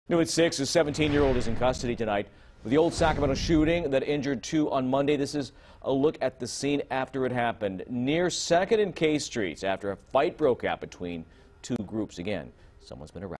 Teen Arrested For Old Sacramento Shooting That Injured 2
The suspect was 17 years old. The two people shot were last reported to be in stable condition.